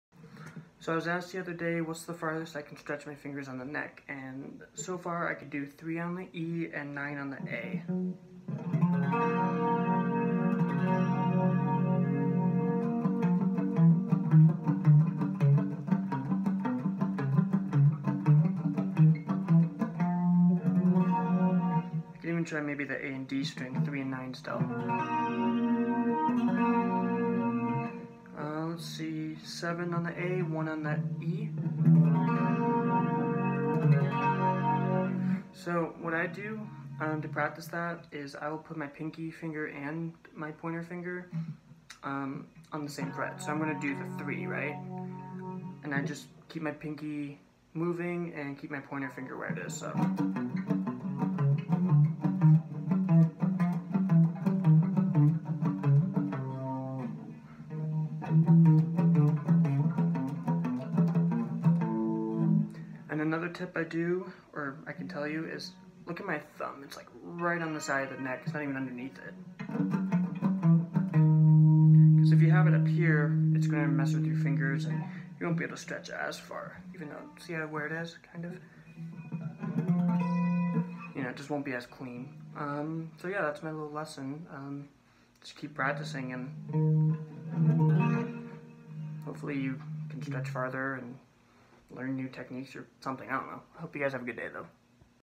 Guitar lesson: Stretch Farther on Fretboard
Just a quick guitar lesson on how I practice stretching farther on the guitar neck. (Currently, I have a 7 fret stretch) hope this video helps :)